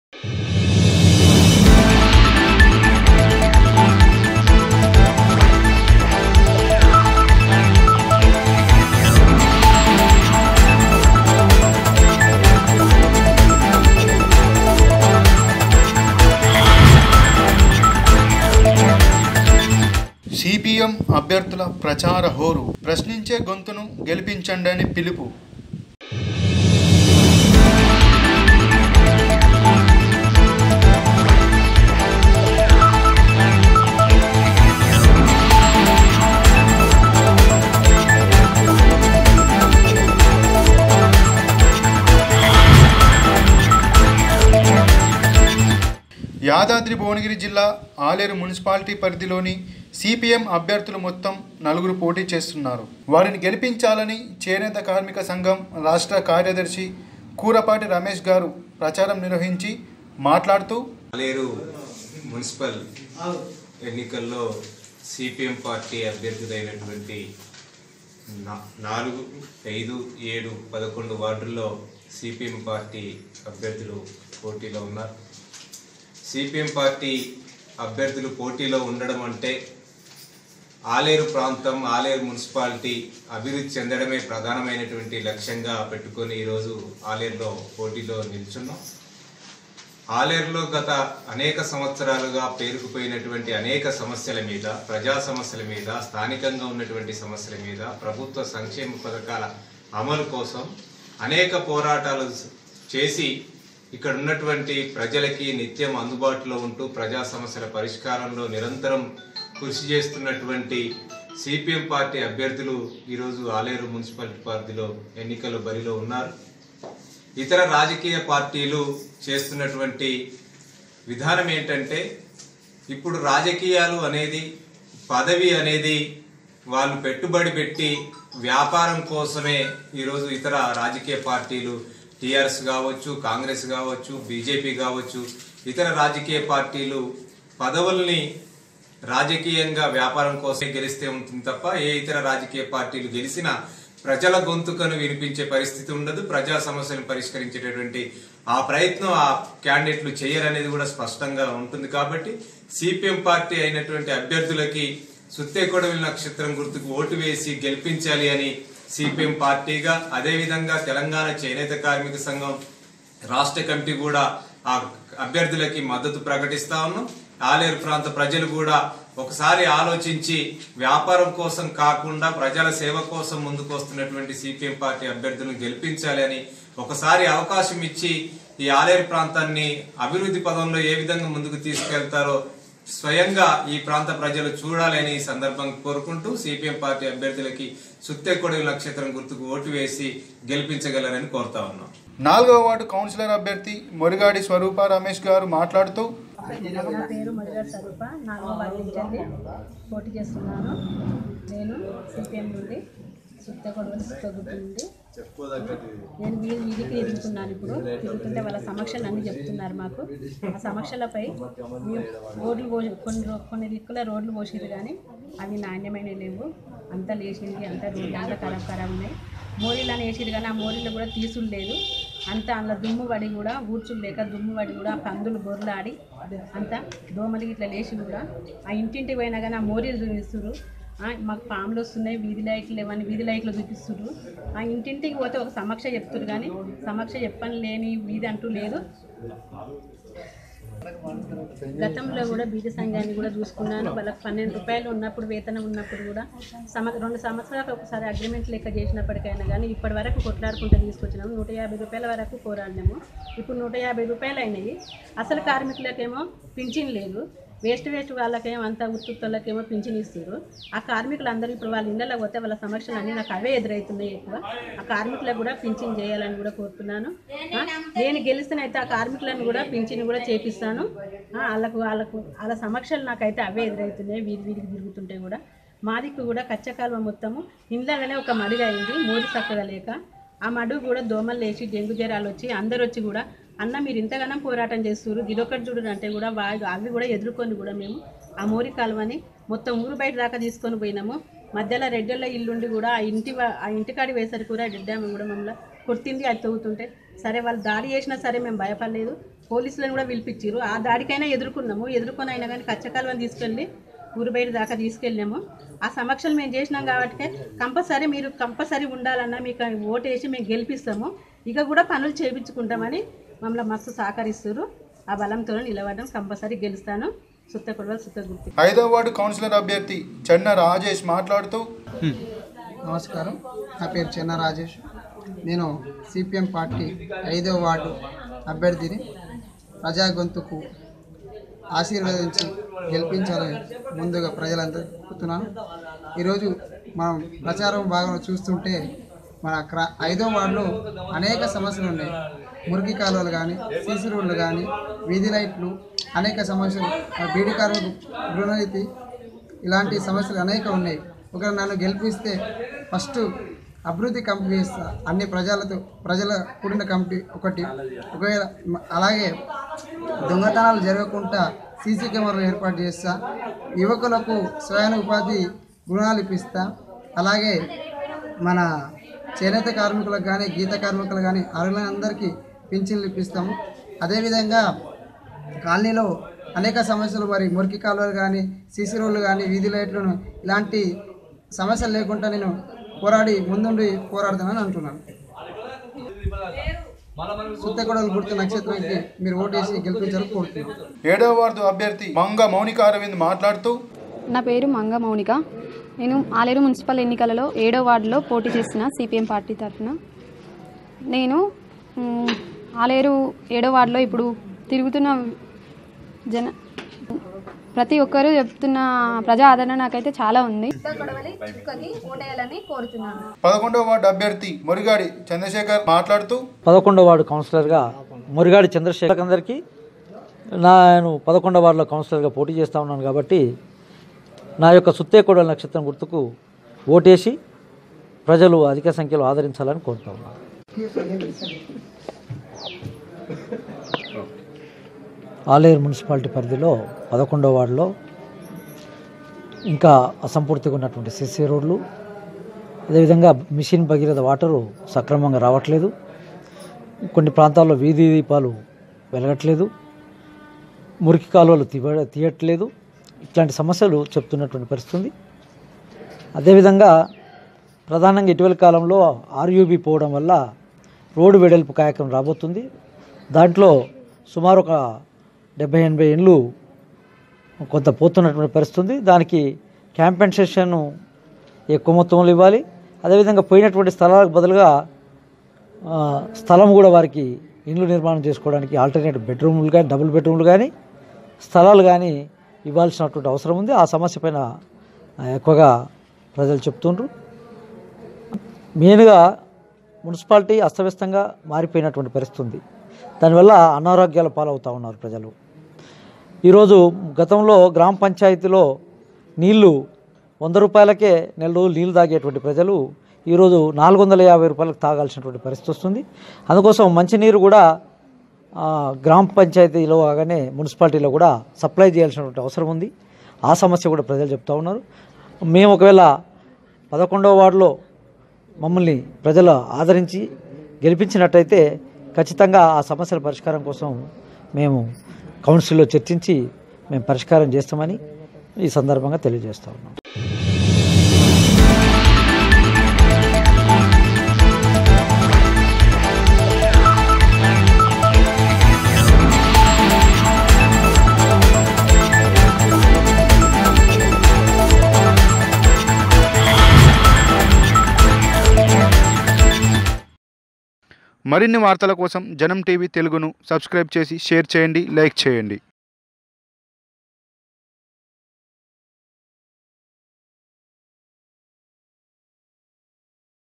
JANAM NEWS CHANNEL || ప్రశ్నించే గొంతును గెలిపించండని పిలుపు || 20.01.2020
Janam NEWS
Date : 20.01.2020

జనం టీవీ  ఆలేరు పుర పోరు ప్రత్యేక కథనానికి స్వాగతంయాదాద్రి భువనగిరి జిల్లా, ఆలేరు మున్సిపాలిటీ పరిధిలోని CPM అభ్యర్థులు మొత్తం నలుగురు పోటీచేస్తున్నారు.